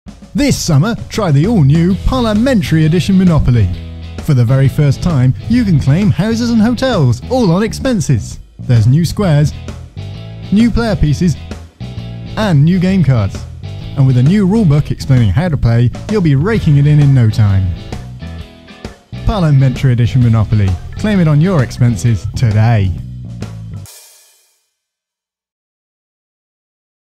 MP Monopoly
The ALL NEW Parliamentary Edition Monopoly!

Who needs to pay rent & mortgage property's when the community chest can pay for it? Live like an MP in this updated version of the classic game.

(Game not actually available. Graphics, script and visuals by Me - Music